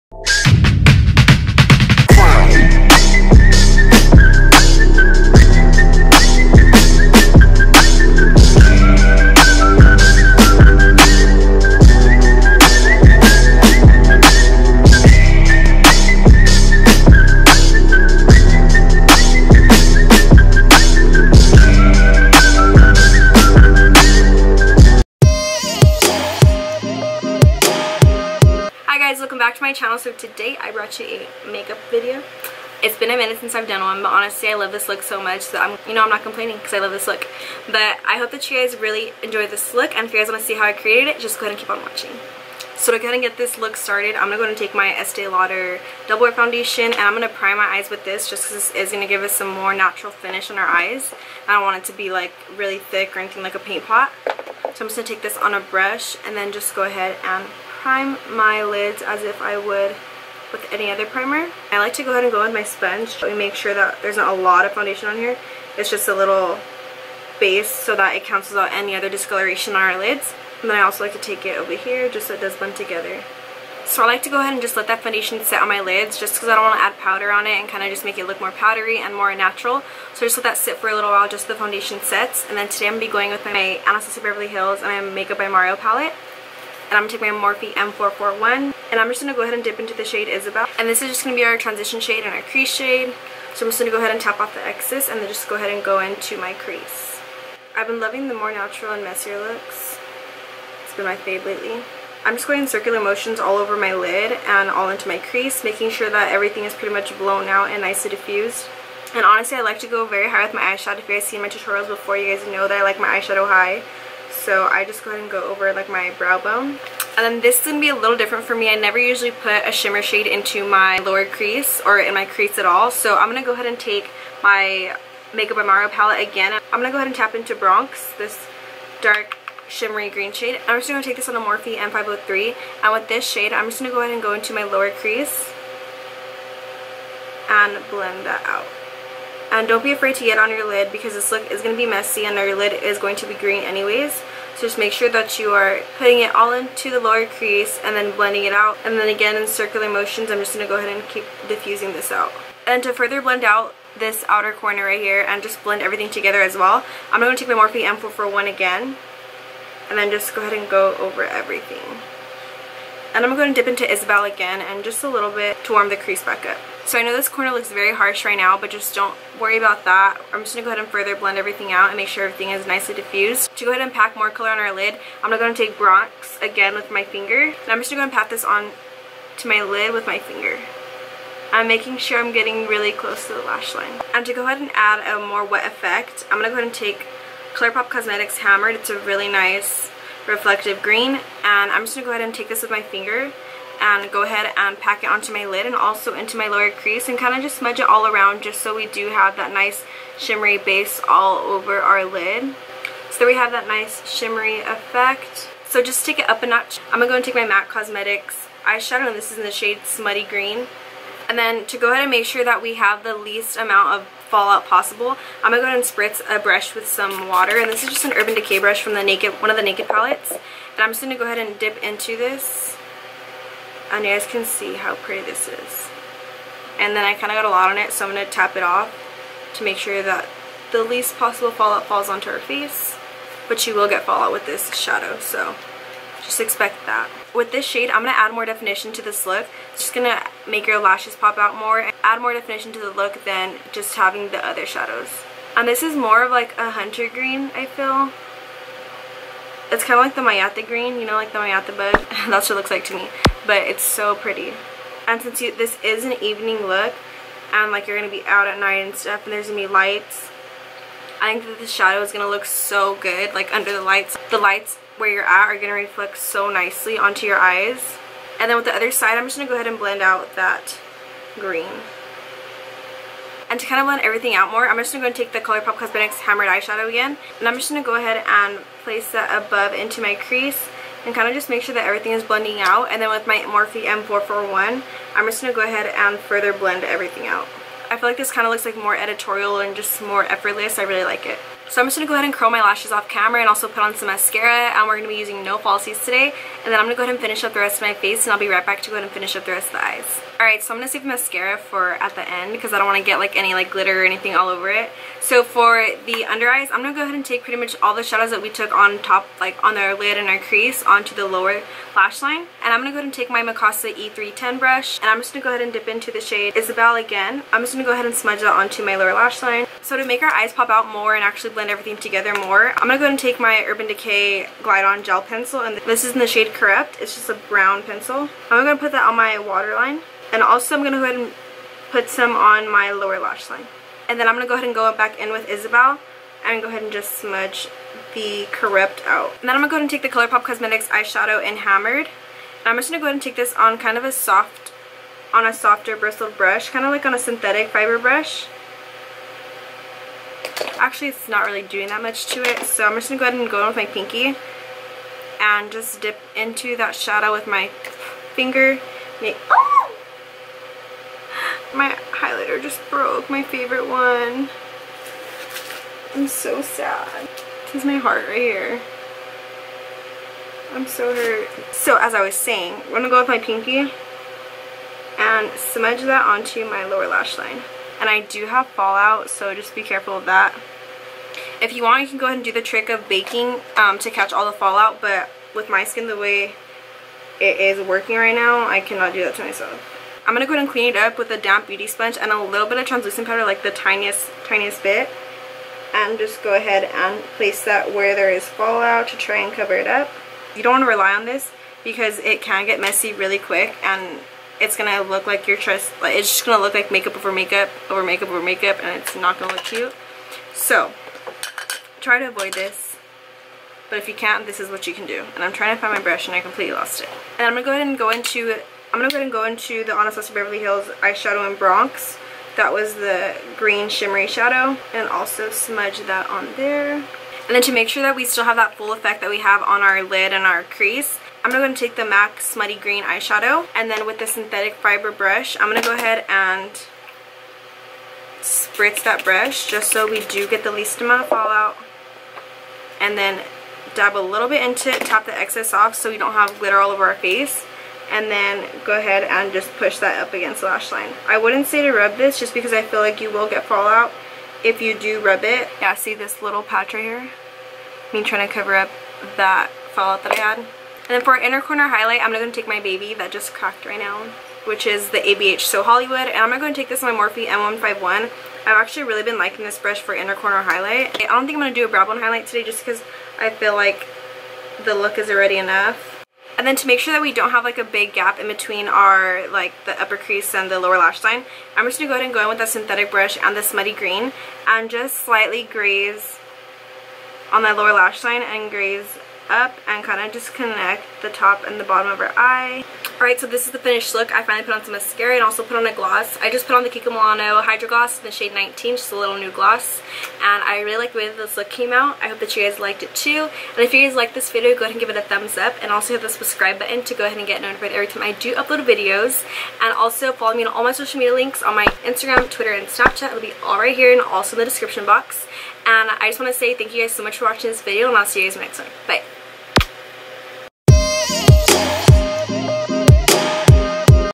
Olive Summer Night Out Makeup Tutorial | Bryana Jordyn
Hi Loves, It's been a while but here's a little summer night out makeup tutorial that I am obsessed with !

Products Used

Estee Lauder Double Wear Foundation
ABH x Makeup by Mario Palette
Colourpop Cosmetics "Hammered" Super Shock Shadow
Mac Cosmetics "Smutty Green" Shadow
Kiko Milano Hydra Gloss "19"